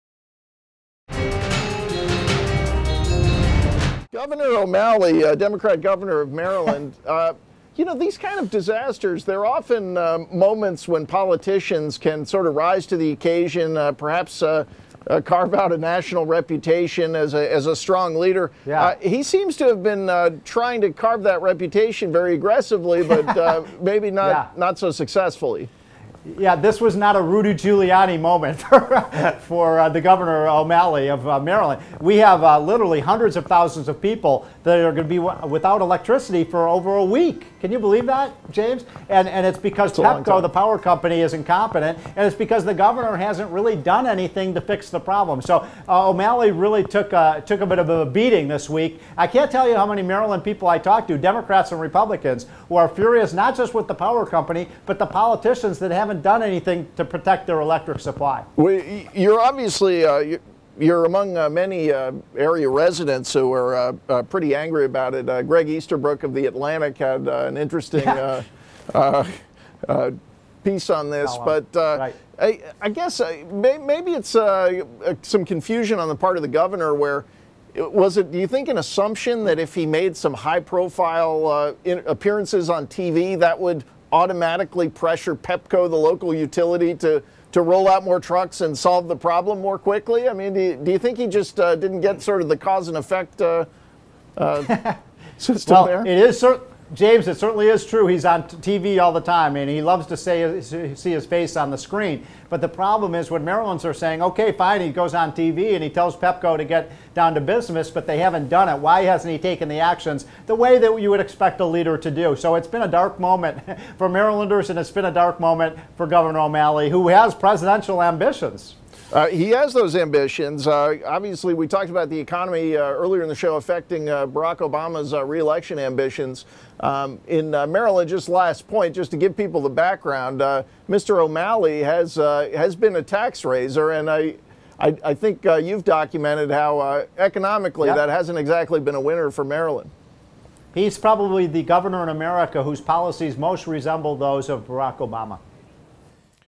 O'Malley's Black Out: WSJ Opinion
Editorial board member Steve Moore on whether the D.C. power outage will hurt Maryland Governor Martin O'Malley's political ambitions. Photo: Associated Press.